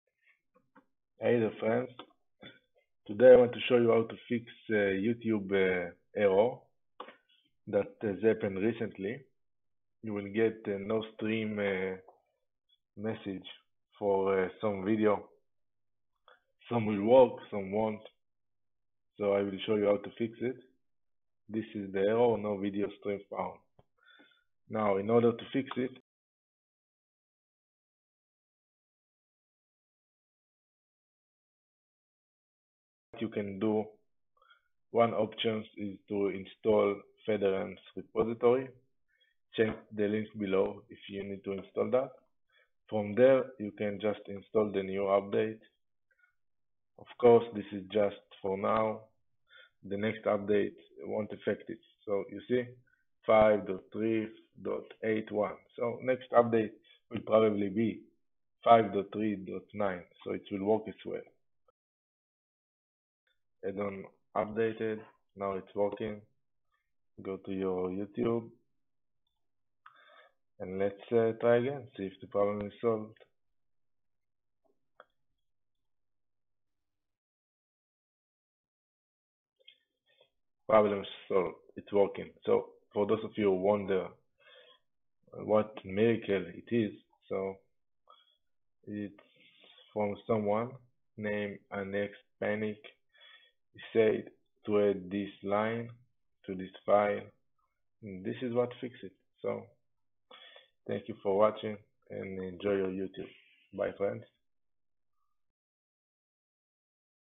YouTube No videos streams found (Kodi)
Hey friends!
An easy fix for No video streams found issue that has occur on Feb, 2017 in YouTube addon (Kodi).

You may download featherence repository by entering the website below and clicking on the featherence icon at the bottom left screen!